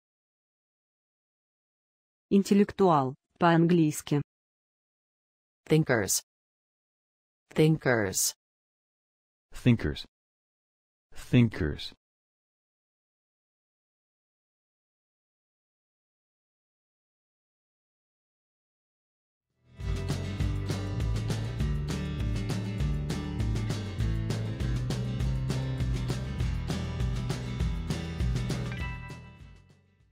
Интеллектуал по-английски
Как правильно сказать Интеллектуал по-английски? Слушай вместе с нами.
А как будет Интеллектуал в переводе с русского на английский? Говори, как произносят американцы. И еще, как правильно написать слово Thinkers?

Спасибо!